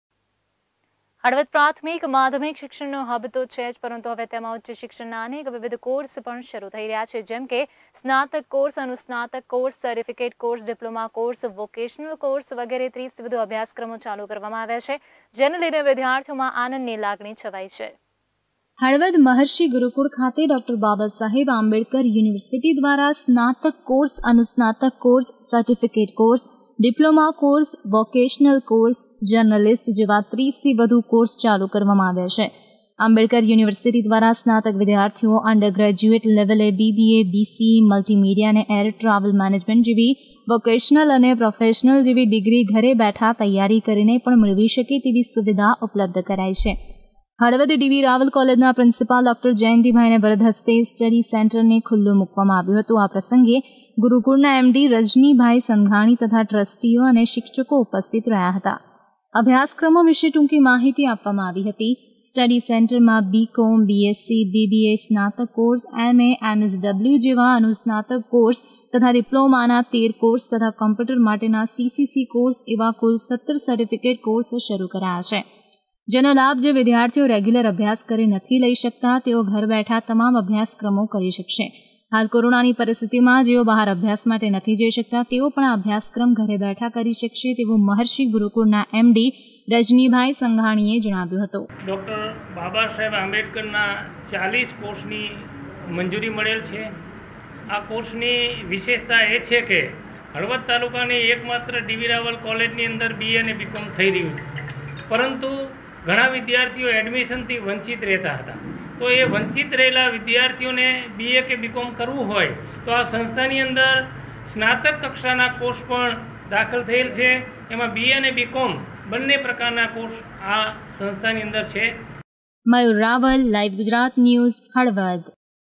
હળવદ : ગુરુકુળમાં આંબેડકર યુનિવર્સિટીના વિવિધ અભ્યાસક્રમો શરુ કરાયા
હળવદ પ્રાથમિક, માધ્યમિક શિક્ષણનું હબ તો છે જ, પરંતુ હવે તેમાં ઉચ્ચ શિક્ષણના અનેક વિવિધ કોર્ષ પણ શરુ થઈ રહ્યા છે. જેમ કે સ્નાતક કોર્સ, અનુસ્નાતક કોર્સ, સર્ટિફિકેટ કોર્સ, ડિપ્લોમા કોર્સ, વોકેશનલ કોર્સ, વગેરે ૩૦થી વધુ અભ્યાસક્રમો ચાલુ કરવામાં આવ્યા છે. જેને લઈને વિદ્યાર્થીઓમાં આનંદની લાગણી છવાઈ છે.